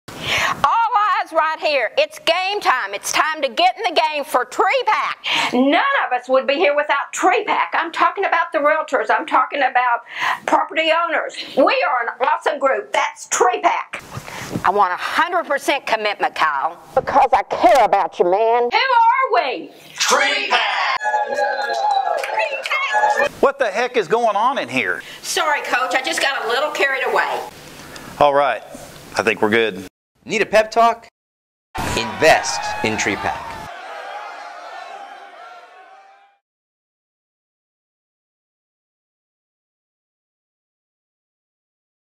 TREPAC funny No. 17: The Fan
The 17th in a series of short videos using humor and role-playing to illustrate the importance of Texas REALTORS® investing in their future through TREPAC.